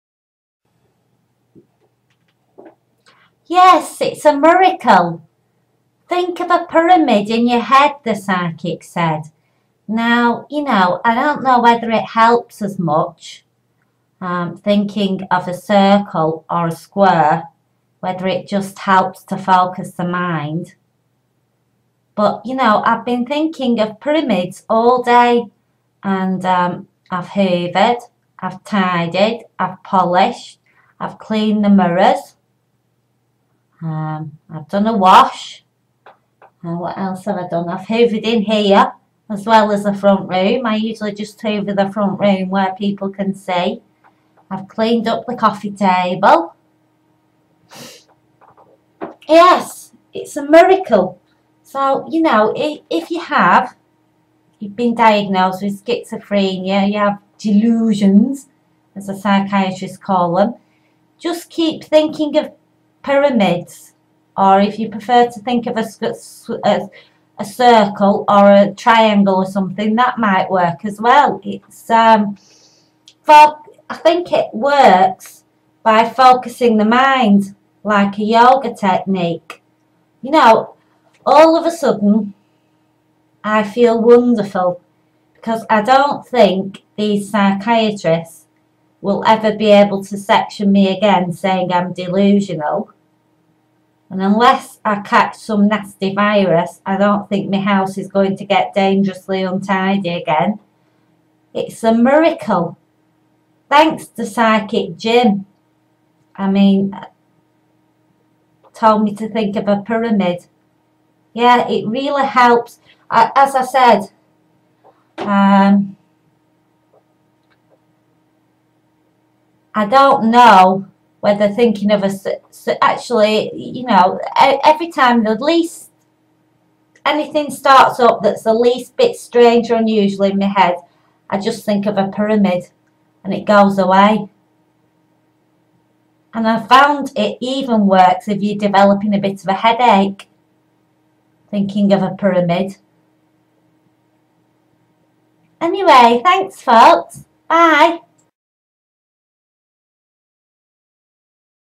It's a miracle
Now when anything strange or unusual starts up in my head I just think of a pyramid and it stops. Weather it justs helps focos the mind I don't know. Maybe thinking of a circle or square will work just as well. I've been pottering around the house since I started doing it looking for things to do. Before I was so exhausted I just used to slob out on the couch or in bed.